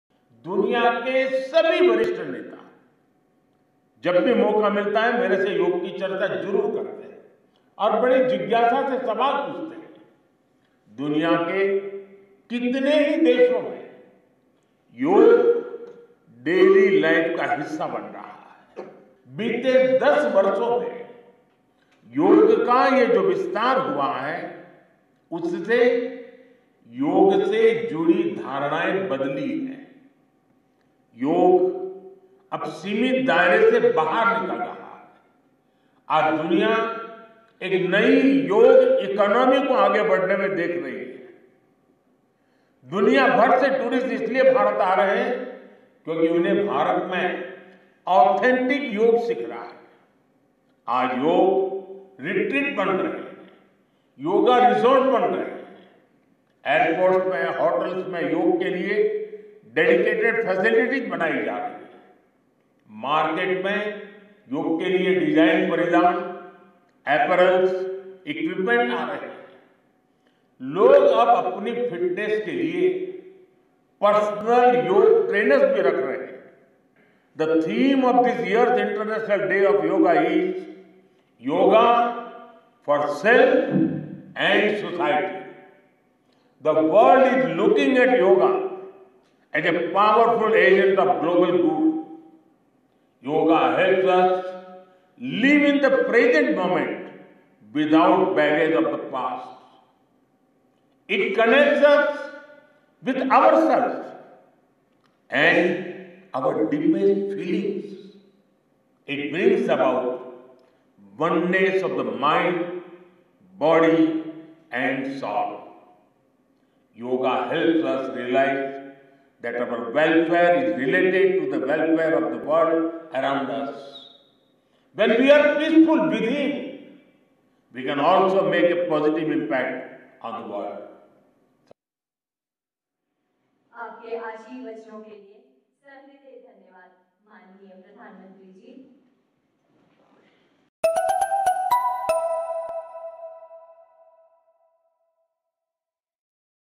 World is looking at yoga as a powerful agent of global good: PM Modi on International Yoga Day
SRINAGAR: Prime Minister Narendra Modi on Friday said the world sees yoga as a powerful agent for global good as it helps people live in the present without carrying the baggage of the past.

Addressing a gathering at the 10th International Yoga Day event at the SKICC here, the prime minister said yoga has helped people realise that their welfare is linked to the welfare of the world around them.

"The world is looking at yoga as a powerful agent of global good. Yoga helps us live in the present moment without the baggage of the past," the prime minister said.

"When we are peaceful within, we can also make a positive impact on the world...Yoga is making new ways of positive change in the society," the prime minister said.

The event was scheduled to be held on the lawns of the SKICC on the banks of the Dal Lake but had to be shifted indoors due to incessant rain.